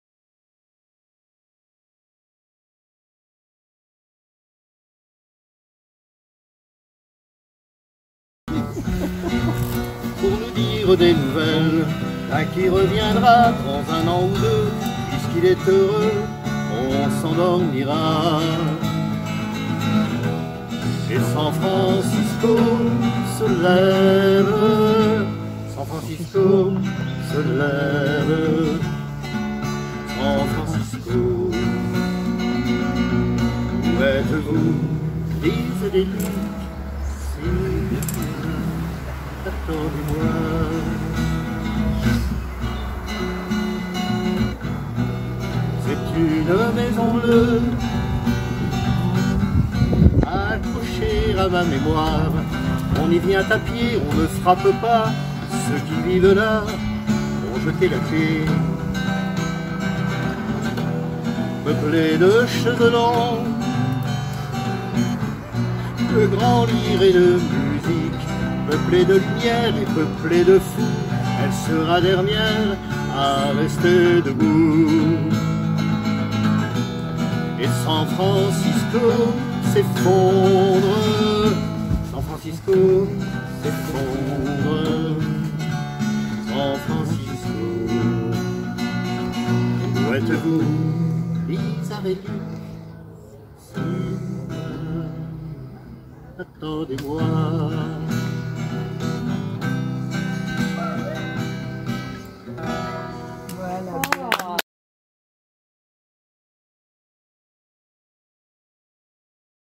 DE LA MUSIQUE SUR UNE AIRE !!!!
DE LA MUSIQUE SUR UNE AIRE ! campingcar vanlife nomade Rencontre magique sur une aire de camping-car. Parce qu'il est des moments précieux, comme celui ci.